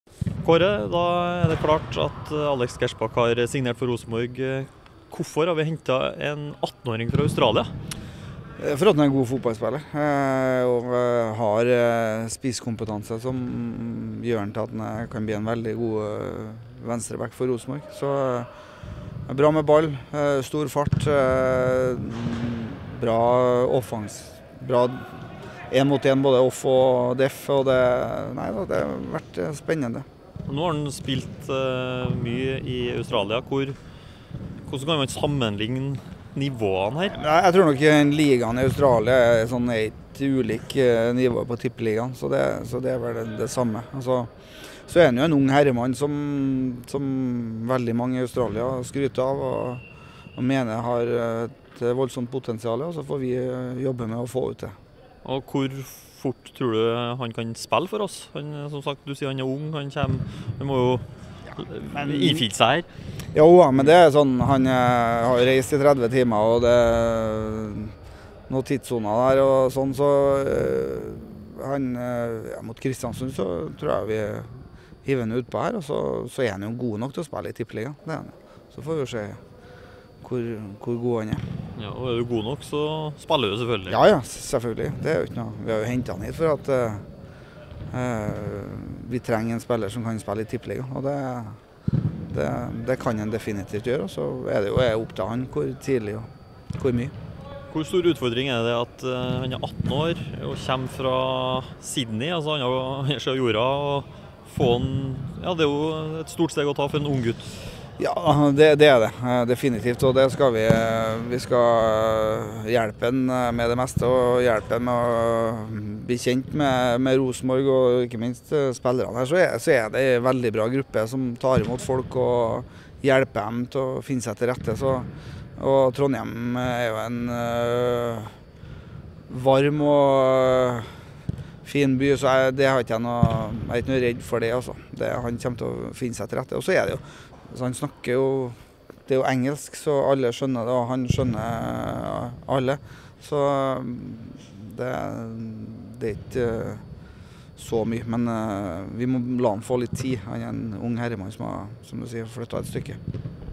- Han har et stort potensiale
Kåre Ingebrigtsen tror Alex Gersbach kan bli en strålende venstreback for Rosenborg.